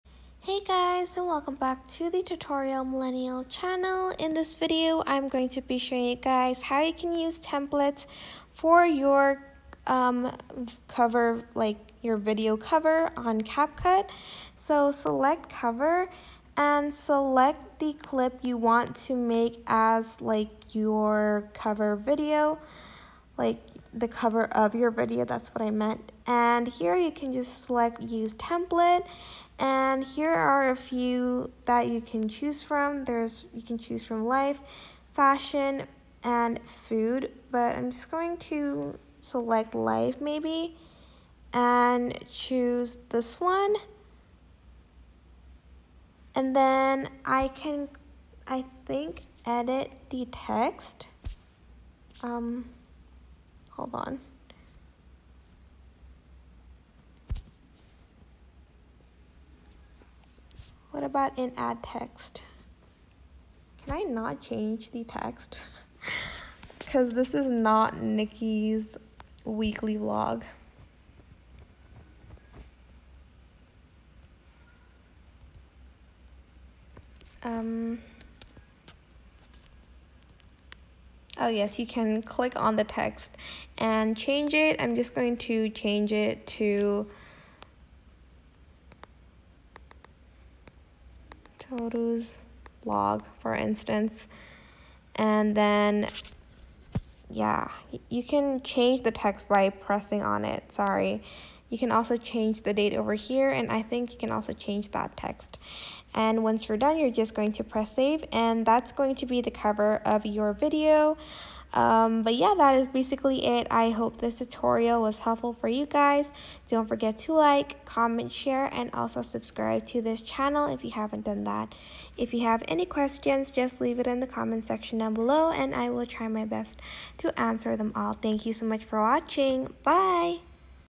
How To Use Cover Video Template On CapCut
This tutorial will show you how you can use cover video templates on CapCut.